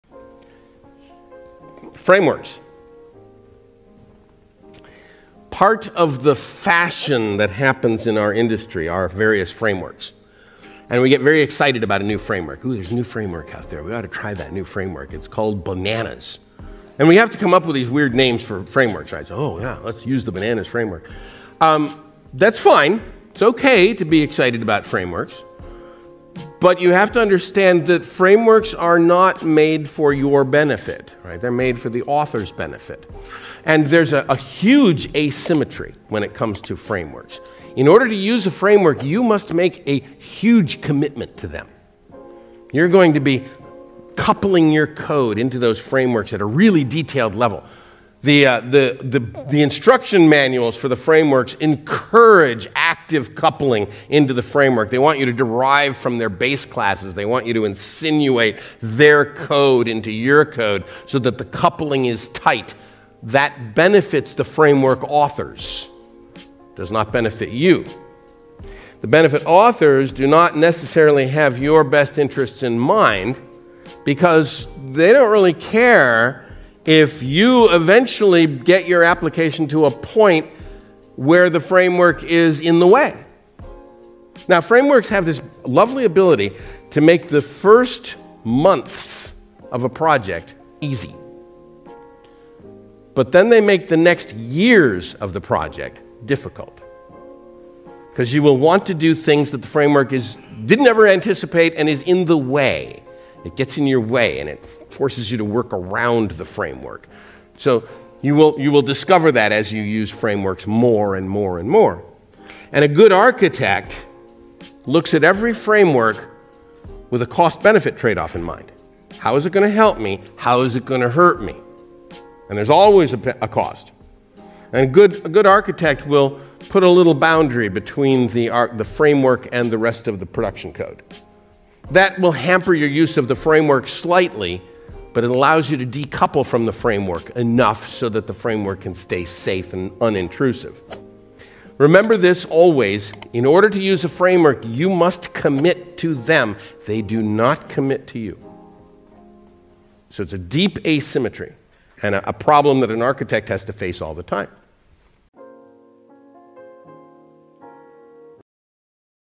The problem with frameworks - Uncle Bob
In this video, Robert C. Martin (Uncle Bob) speaks on the asymmetry between developers and the frameworks they often rely on to do their day to day work.
He brings to mind the level of coupling and commitment that is often encouraged by frameworks, and how these benefit the framework authors while cripling developers in the long run. thoughts?